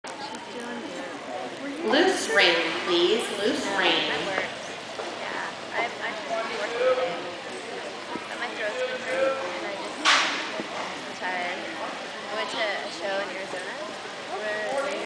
Monique and Becky at Silver Dollar Show Jan 2009
Western pleasure class at the silver dollar show, Albany OR